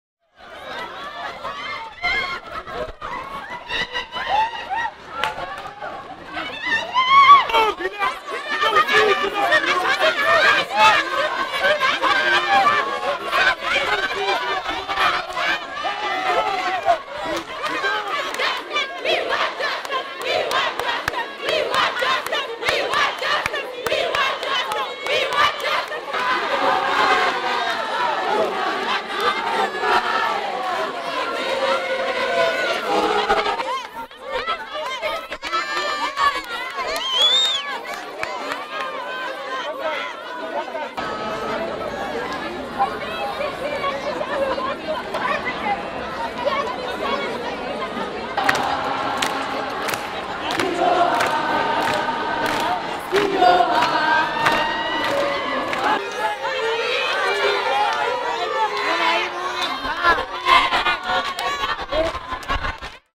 Des manifestants contre les féminicides perturbent le Forum économique mondial au Cap [No Comment]
Des centaines de militants ont protesté contre le meurtre et le viol de femmes en Afrique du Sud au début du Forum économique mondial sur l'Afrique tenu au Cap. Les protestations ont été suscitées par le meurtre récent de deux femmes - une étudiante de l'Université du Cap, Uyinene "Nene" Mrwetyana et la championne de boxe, Leighandre "Baby Lee" Jegels.…